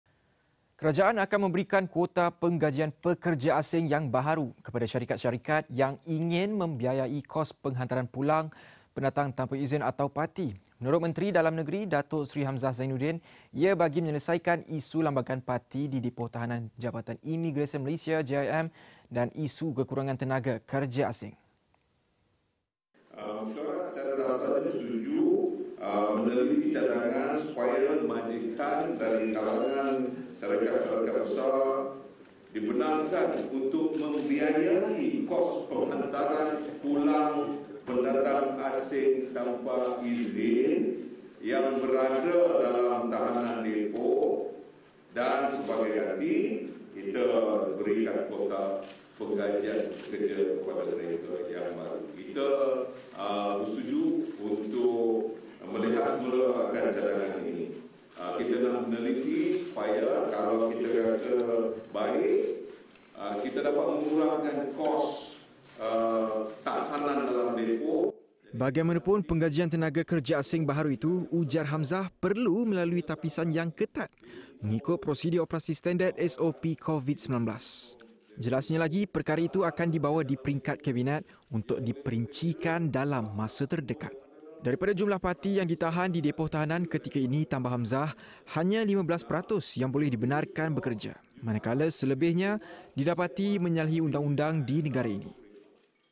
Syarikat biayai kos penghantaran pulang PATI akan diberi kuota
Syarikat-syarikat biayai kos penghantaran pulang PATI akan diberi kuota penggajian pekerja asing baharu
Kerajaan bersetuju untuk memberikan kuota penggajian pekerja asing baharu kepada syarikat yang ingin membiayai kos penghantaran pulang tahanan warga asing di depoh tahanan Jabatan Imigresen Malaysia (JIM).